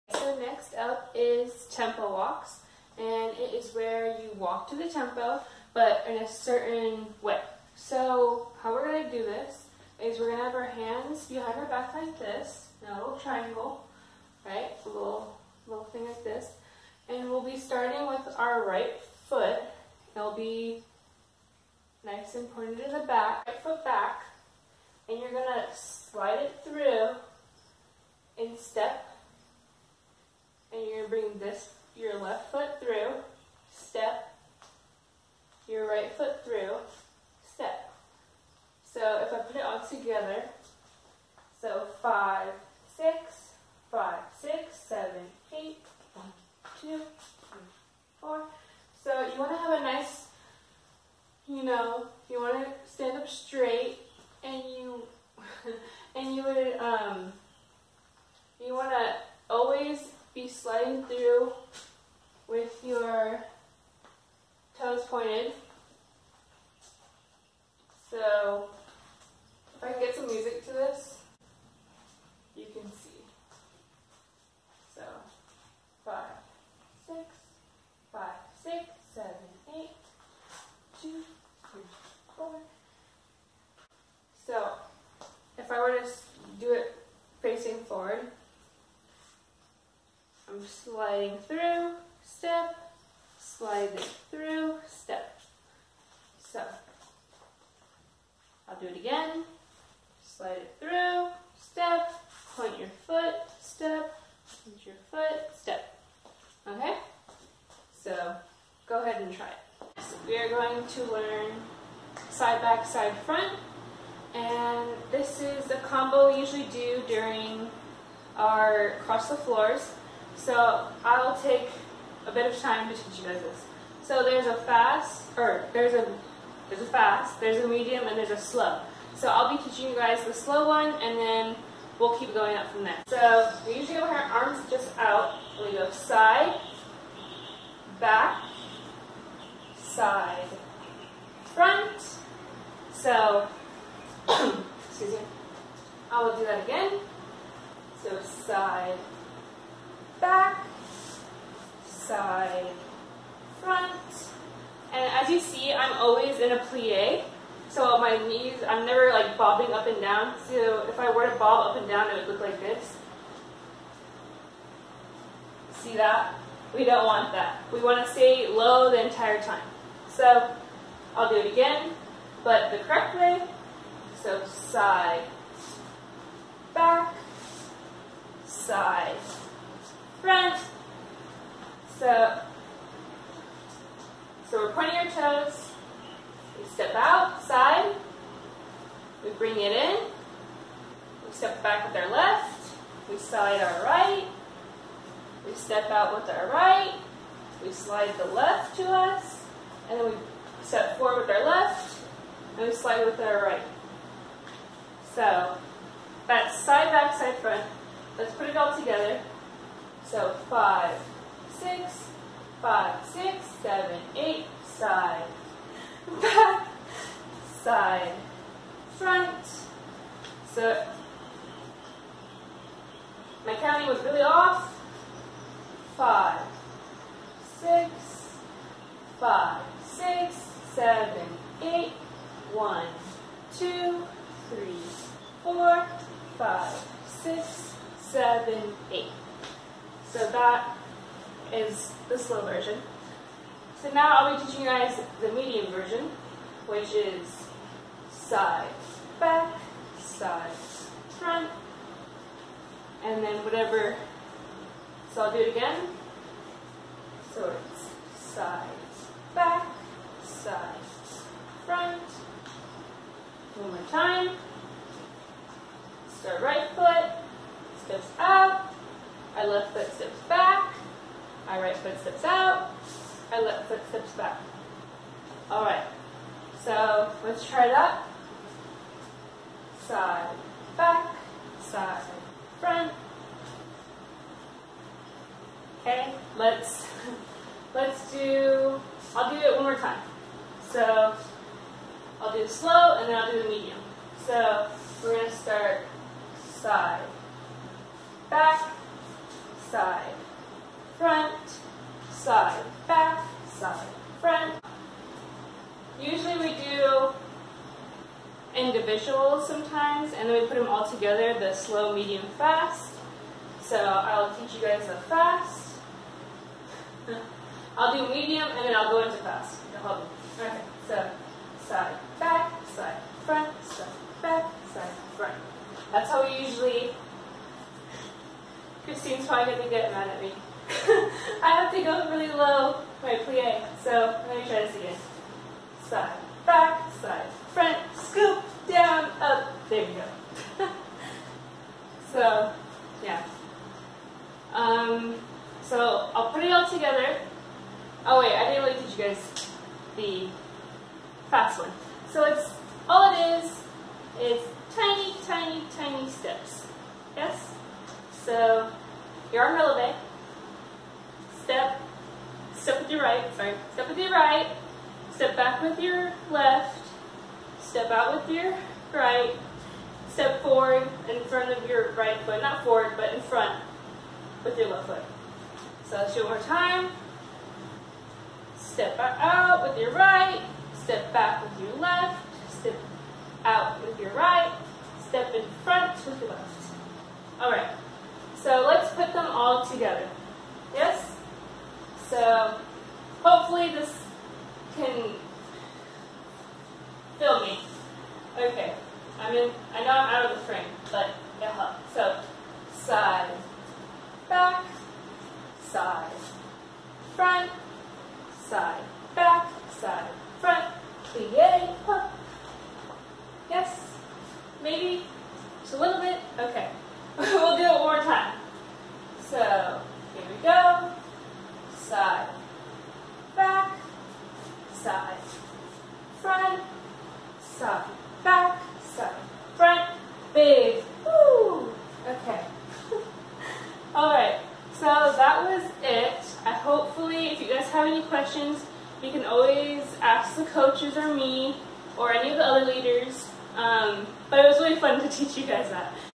WHS Color Guard Dance Basics - Tempo Walks and Side-Back-Front
Taught by WHS Color Guard Dance Captain, Liliana Perez